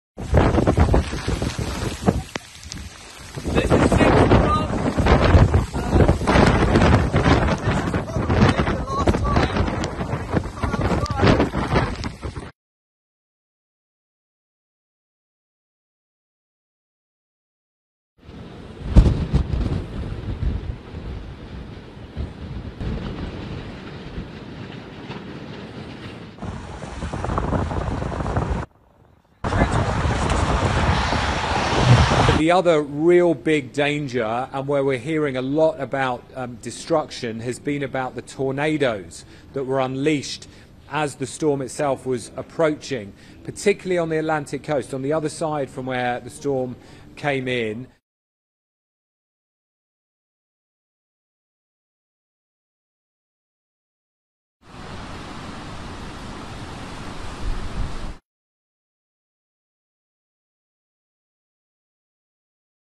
Stadium roof ripped off and other Milton damage
Widespread damage as Hurricane Milton barrels through Florida.

Social Web Links:-

Wait till the next video. Thanks